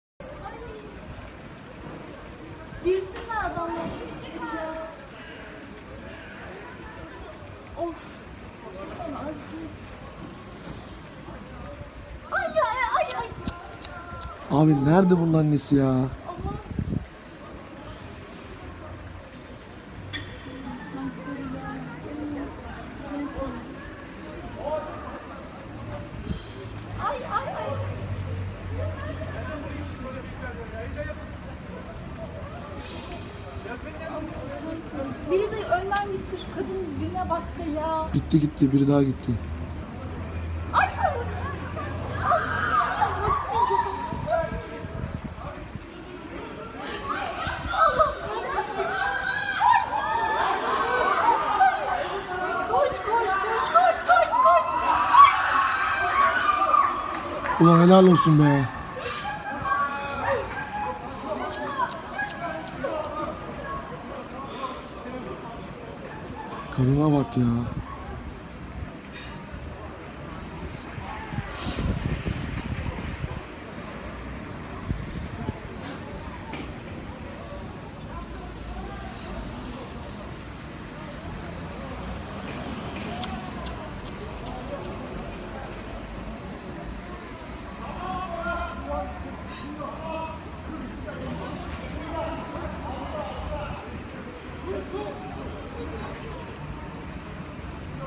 Incredible: Two Year Old is Caught by Neighbors After Jumping out of Third Story Window
This is the tense moment where a two-year-old boy is caught in a blanket by neighbours after jumping from a three-storey apartment building.

The terrifying scene was caught on camera by a passer-by in the Kağıthane district of Istanbul in Turkey.

Civilians, successfully caught the child who had reportedly been hanging out of the window for ten minutes before concerned residents raised the alarm.

Neighbours reportedly tried to yell and scream to get the attention of the mother who was inside the apartment but didn't get any response.

The dramatic video, shot near the scene, shows resourceful residents opening a piece of bed linen under the window to break the child's fall as the toddler clings to the window sill and then finally falls into the blanket.

At that moment, a woman emerges from the window adjacent and alarmed, seems to run down downstairs to the baby's aid. A second person can also be seen emerge from the window in which the baby fell.

The two-year-old was miraculously unhurt after the jump.

Gurkay Guney, a neighbour who rushed to the scene, said the child tried to climb back inside after coming out completely but he failed and let himself fall.

'We tried to calm him down, gave him some water and returned him to his family. He was terrified,' Guney said.

Another resident who witnessed the incident said the mother 'only came downstairs after her child was rescued'.

The family have declined to speak to the press.